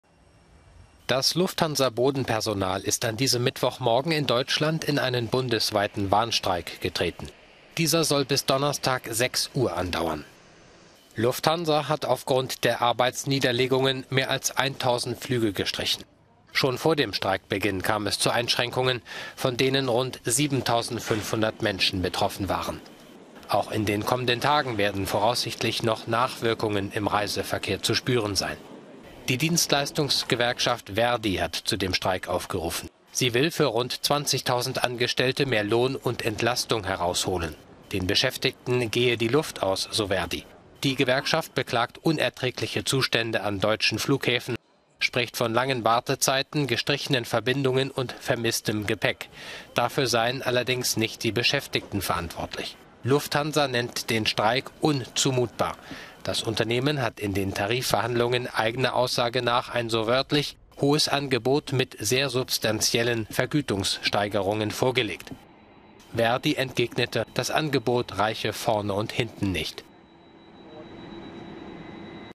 Streik bei Lufthansa: Deutscher Luftraum am heutigen Mittwoch ausgedünnt
100 Flüge wurden gestrichen.